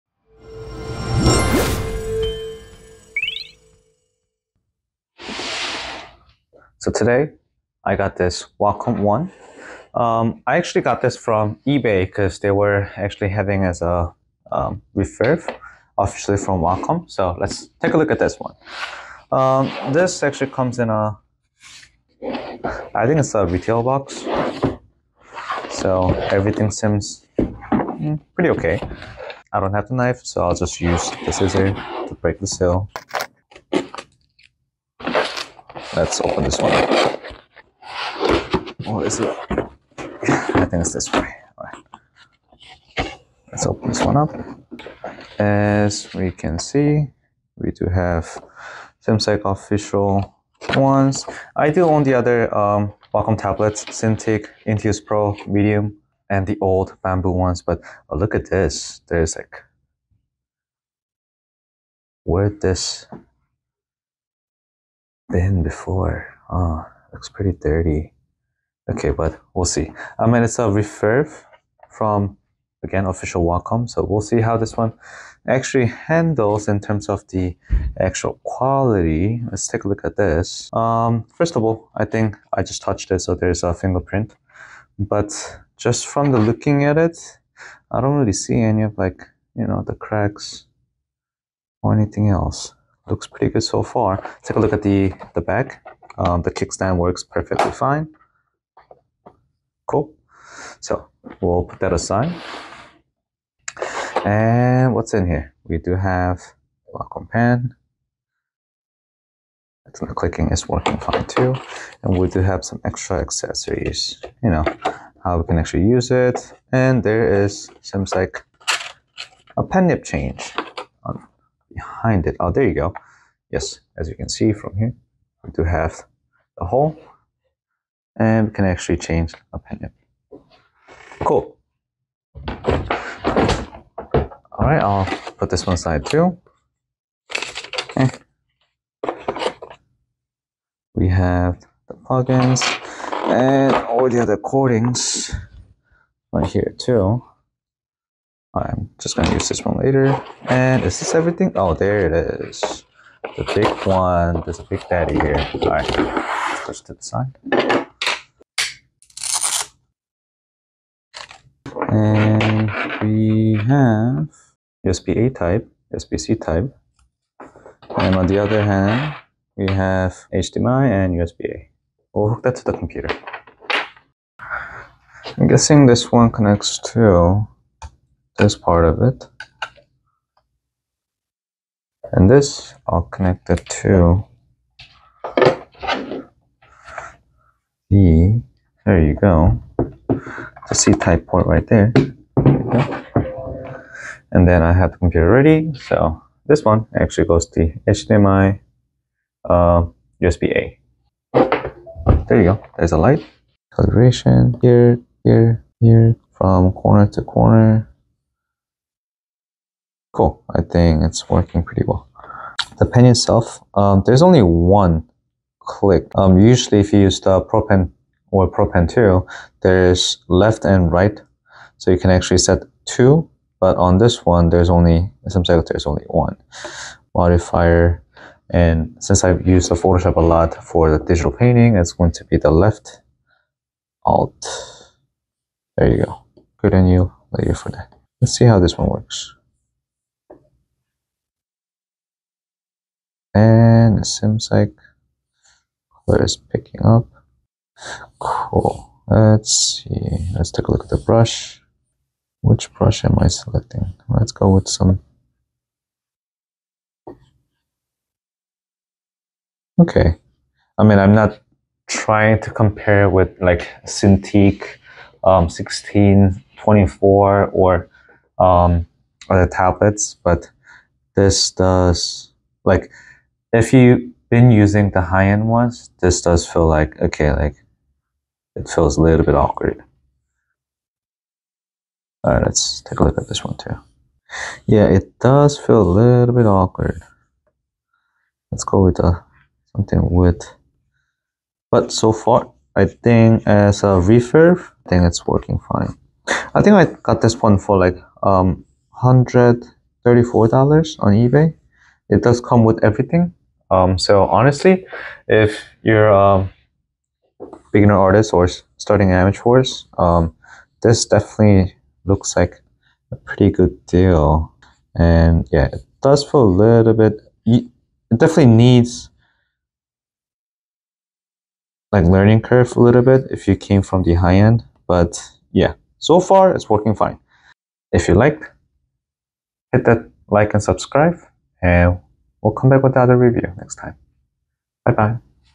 Used Wacom One tablet from Ebay...Worth it?
Today I got used Wacom One from Ebay Wacom Official Outlet.
Let's see if the product will hold it's value even as a used tablet.

Links to the product

What product should I review next?